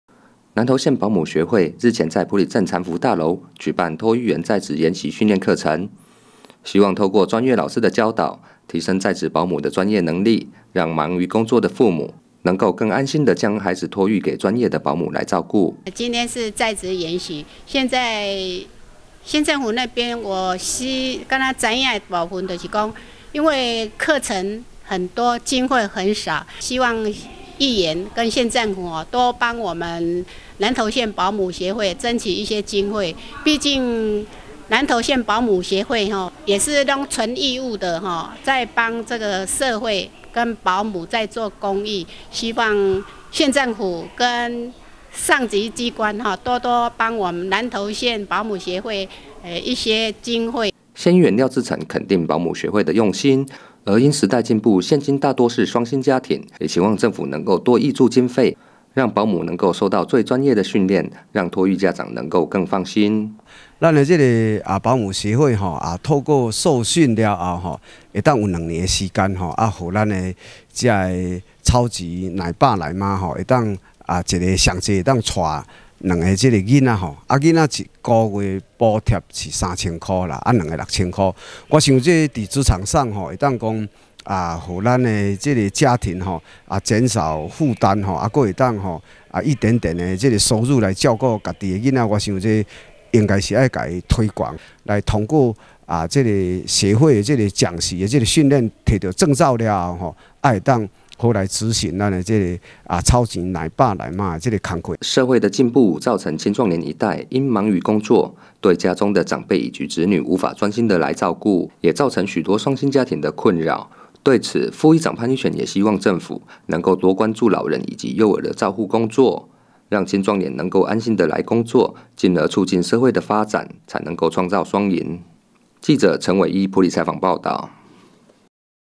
20170829 民議新聞 在職褓母研習課程 提升專業能力讓托育父母安心(縣議員 廖志城)
南投縣褓母學會日前在埔里鎮殘福大樓舉辦托育員在職研習訓練課程，希望透過專業老師的教導提升在職保母的專業能力，副議長潘一全肯定褓母學會的用心，也期望政府能夠多關注年長者及幼兒照護的問題，讓青壯年一代能夠安心的工作為社會進步共同盡一份心力。